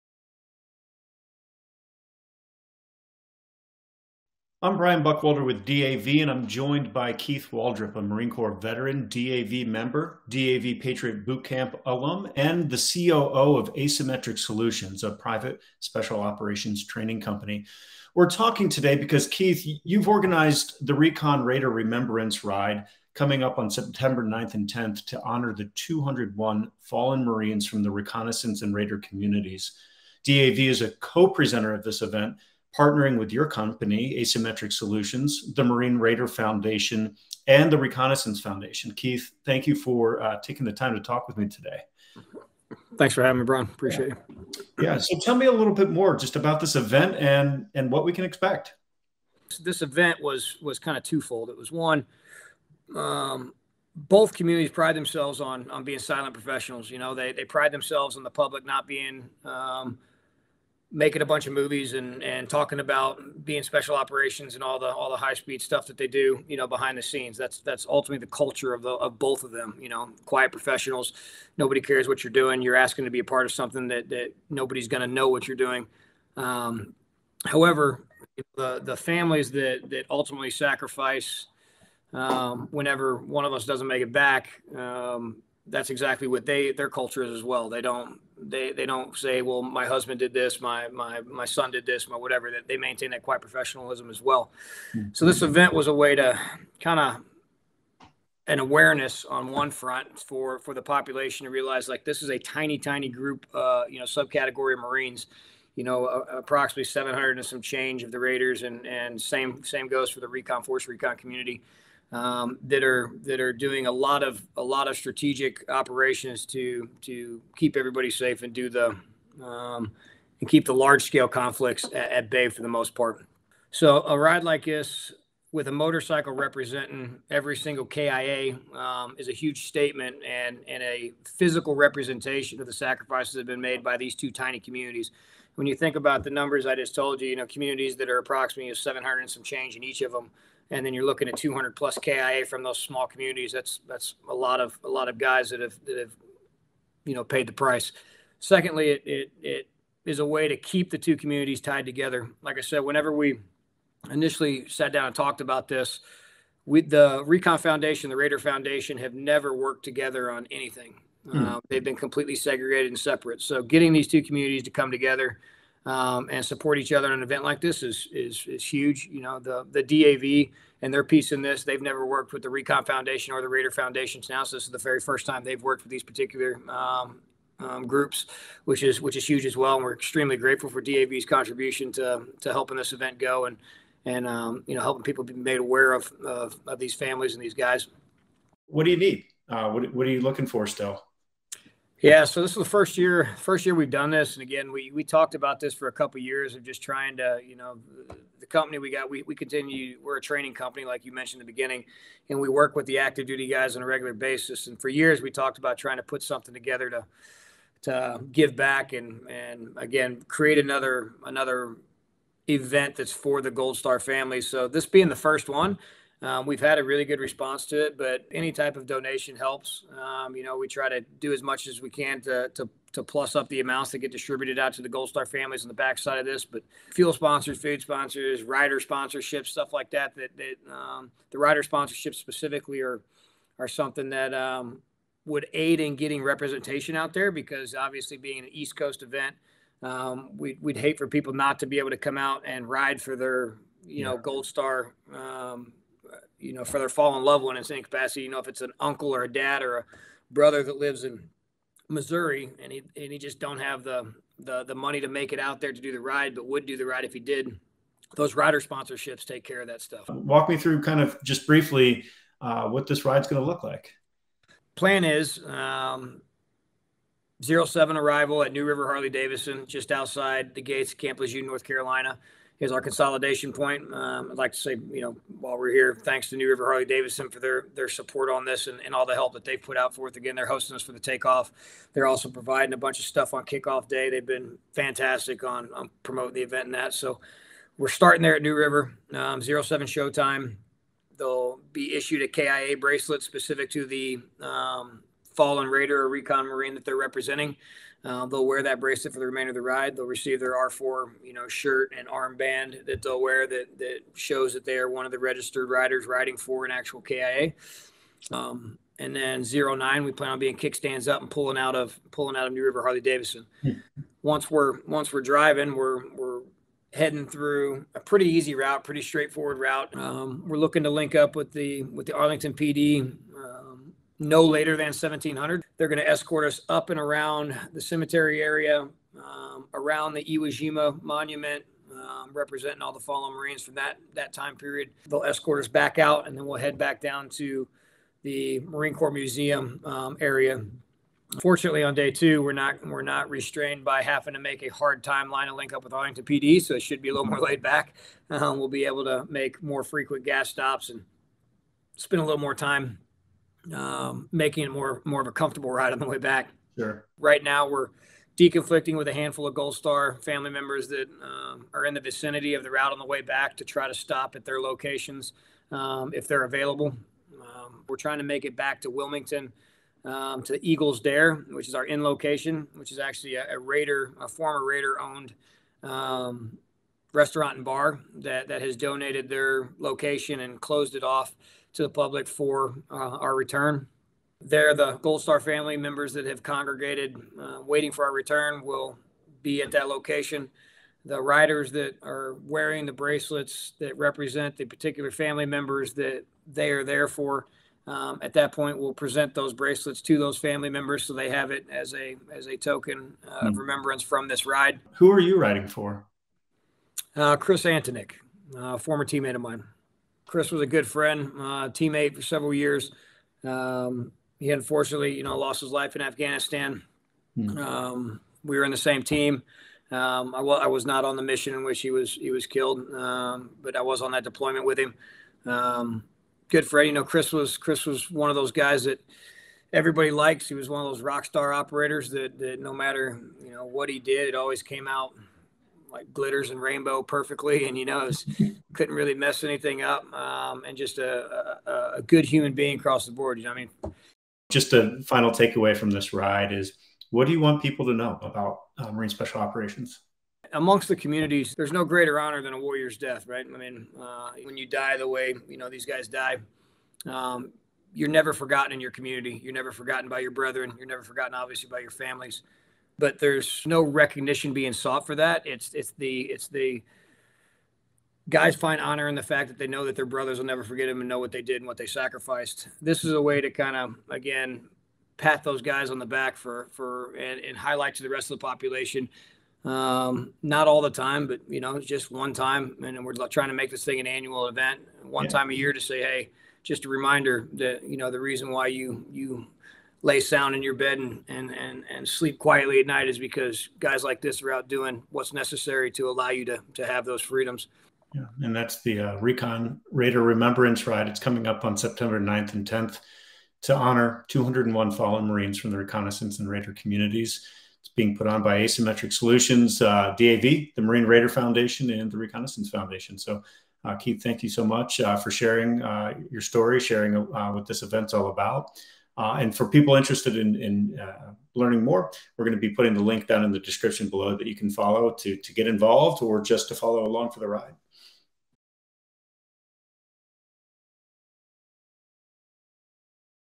Recon Raider Remembrance Ride - Sept. 9-10, 2023
On Sept. 9-10, DAV is co-presenting the inaugural Recon Raider Remembrance Ride with Asymmetric Solutions, the Marine Reconnaissance Foundation, and the Marine Raider Foundation. The ride will honor to lives of the 201 Marines in the special operations community who have fallen in service to their country. We talk with Keith Waldrop, the event’s organizer about the ride’s importance and what we can expect.

to learn more and to support.